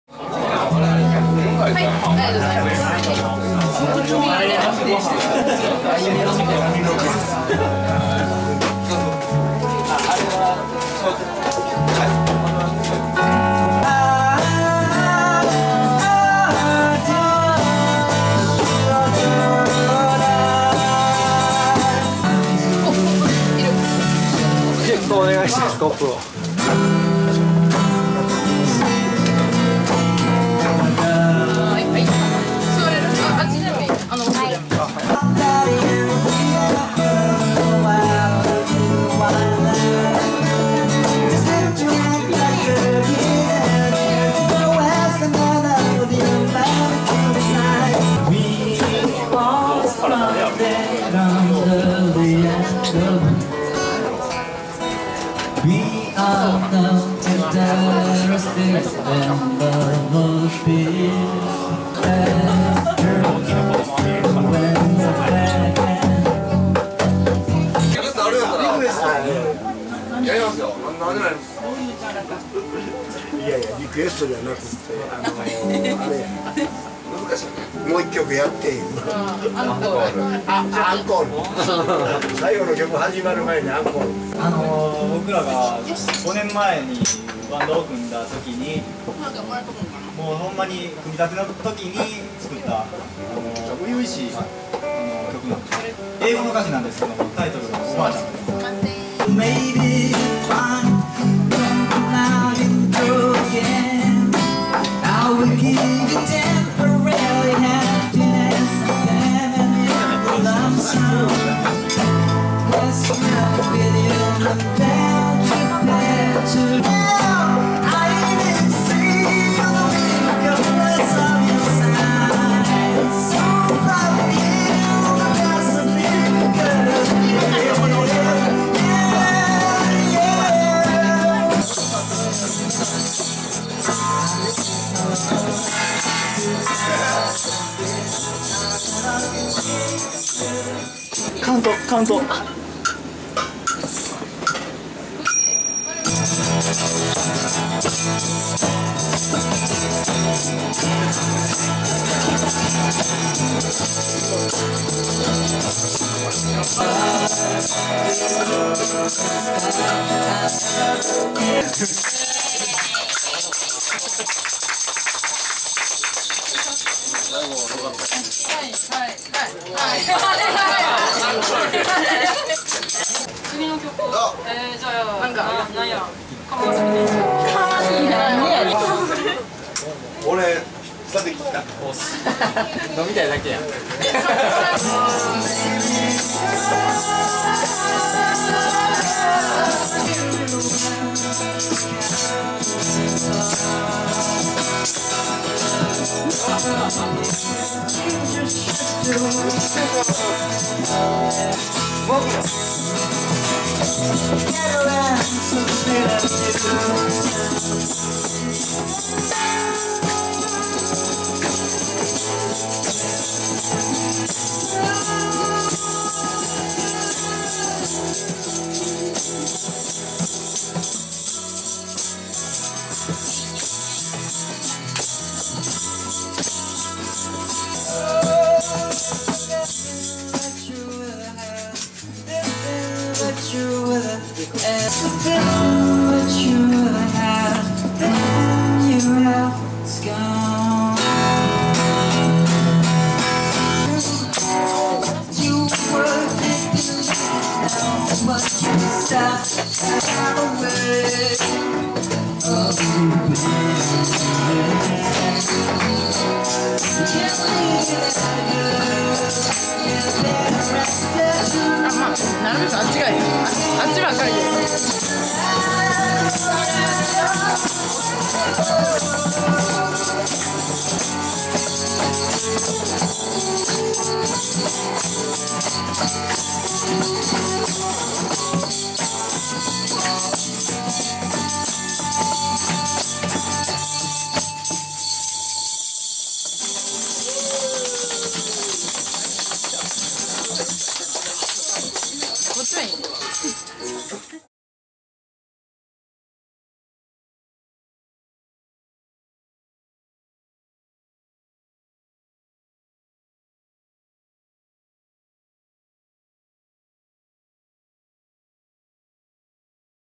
ココルームお座敷ライブ（夕焼けにゃんにゃん）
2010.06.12　ココルームで行われた夕焼けにゃんにゃん（バンド）のライブの模様

プロデュース・タコさん

企画　アッコ、onoomer